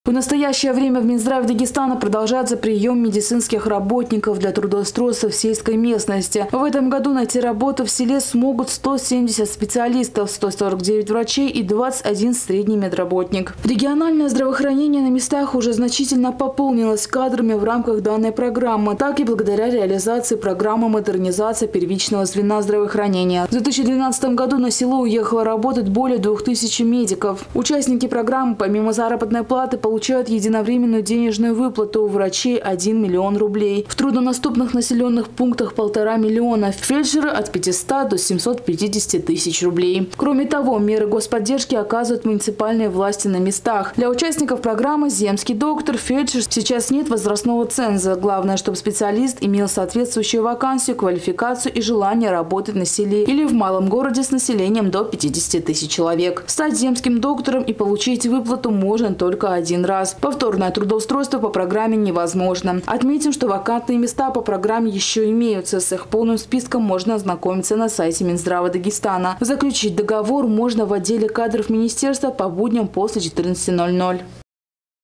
170 медработников в текущем году трудоустроят в дагестанских сёлах по программе «Земский доктор»
В настоящее время в Минздраве Дагестана продолжается приём медицинских работников для трудоустройства в сельской местности. В этом году найти работу на селе смогут 170 специалистов - 149 врачей и 21 средний медработник. Региональное здравоохранение на местах уже значительно пополнилось кадрами как в рамках данной программы, так и благодаря реализации Программы модернизации первичного звена здравоохранения. С 2012 года на село уехали работать более 2000 медиков.